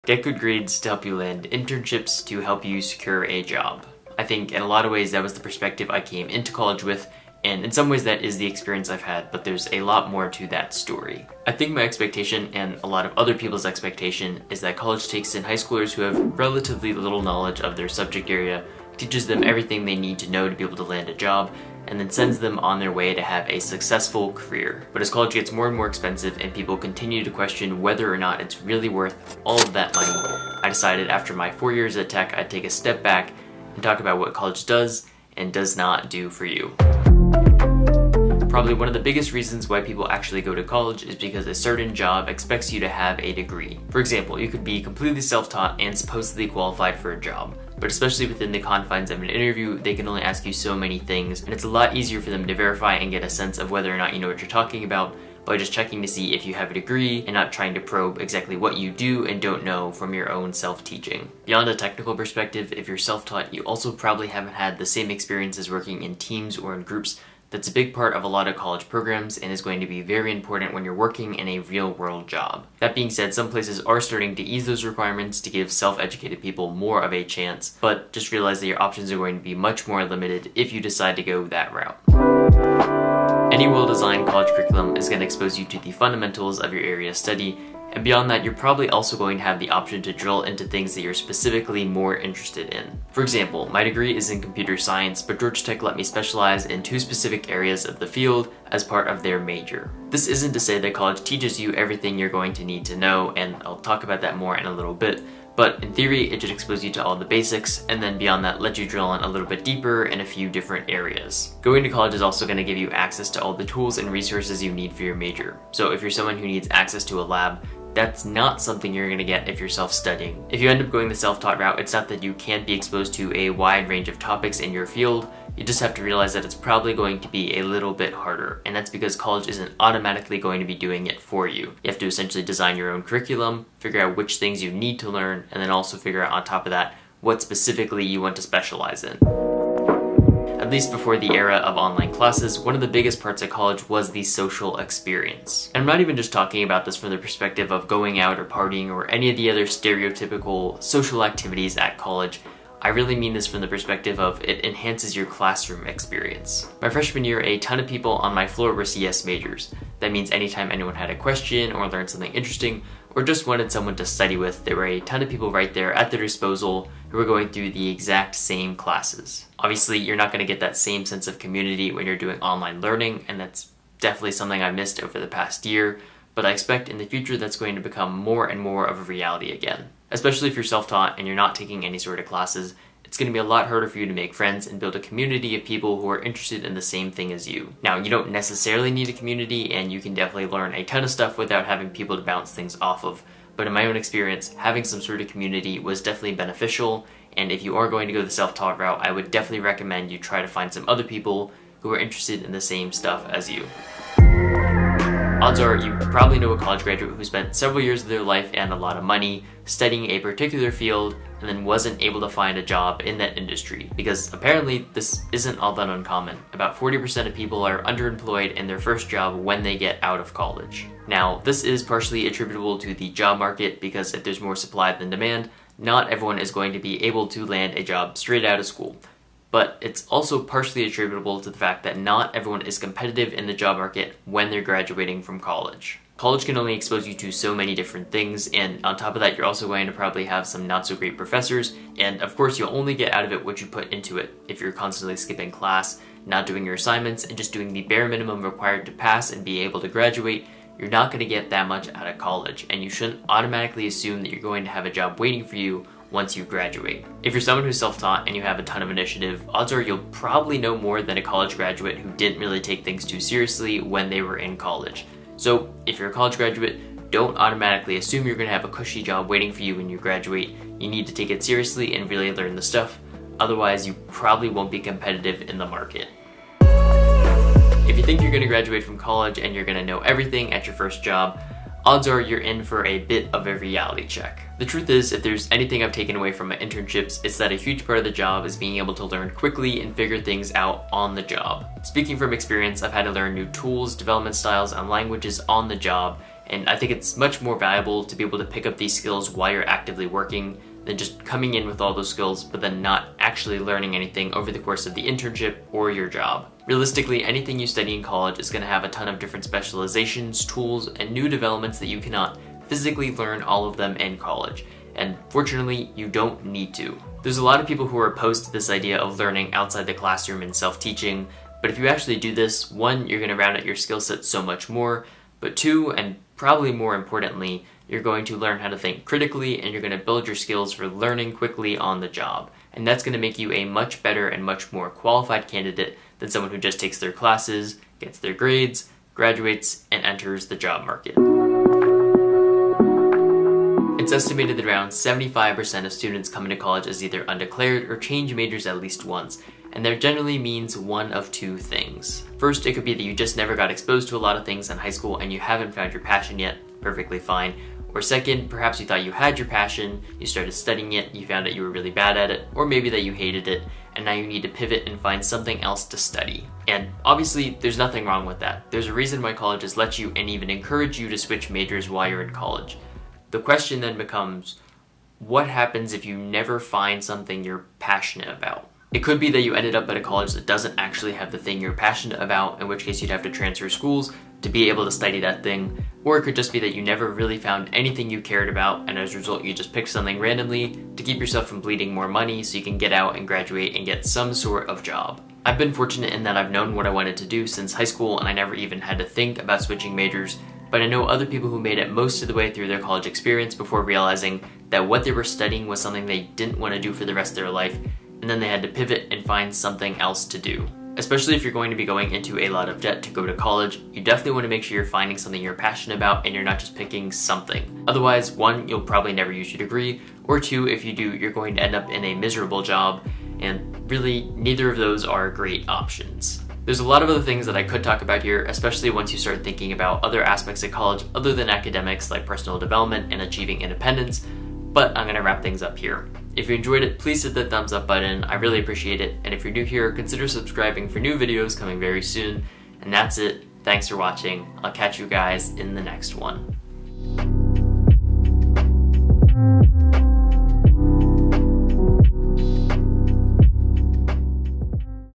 What I Got Wrong About My College Degree
After four years of college, I decided to look back at what a college degree does and does not do for you.

✍️ Looking for more software engineering content?